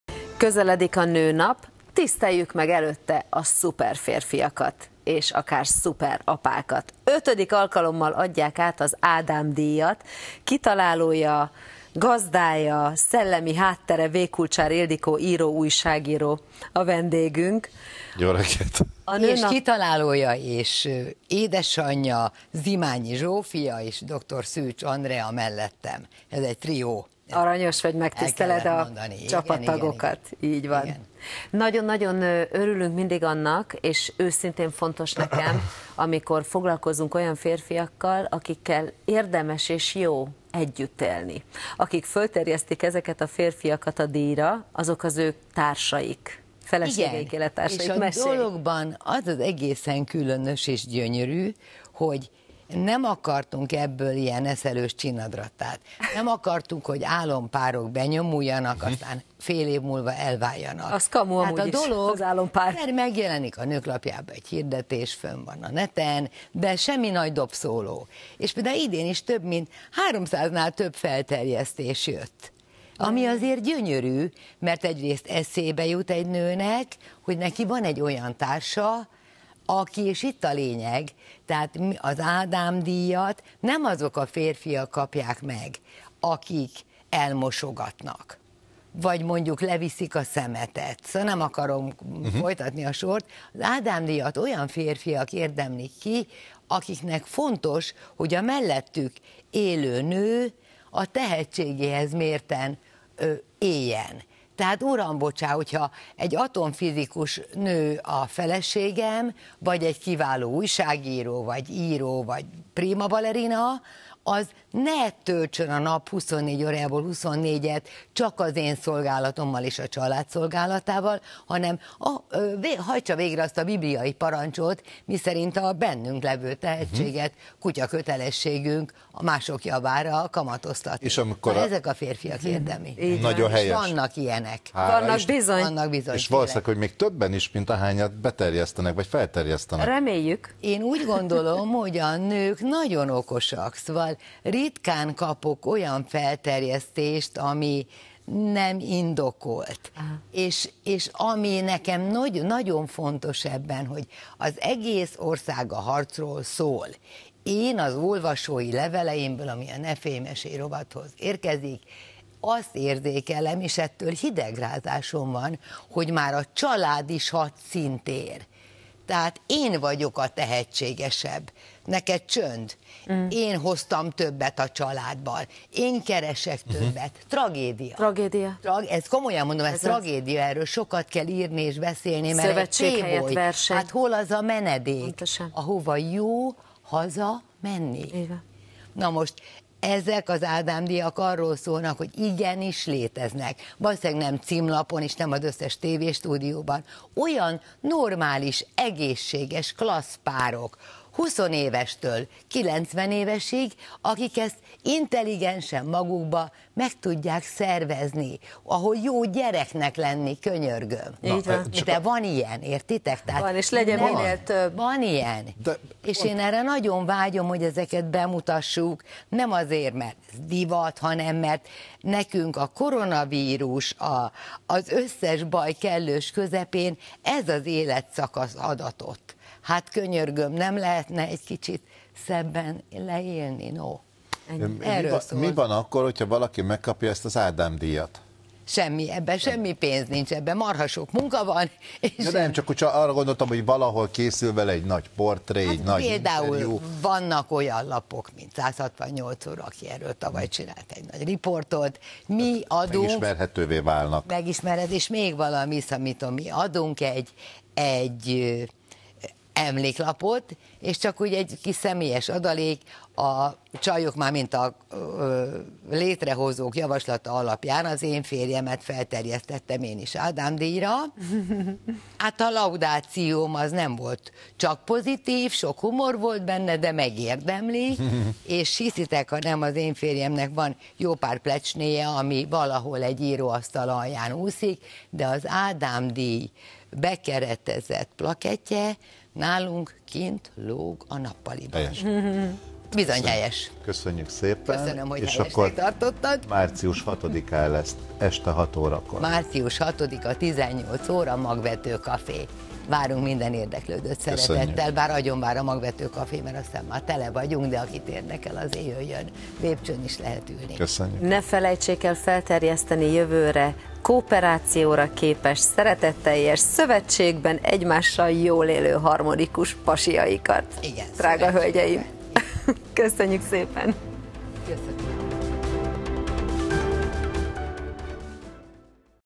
Nőnap előestéjén ötödik alkalommal adják át az Ádám-díjat
A Start Plusz vendége V. Kulcsár Ildikó író, újságíró.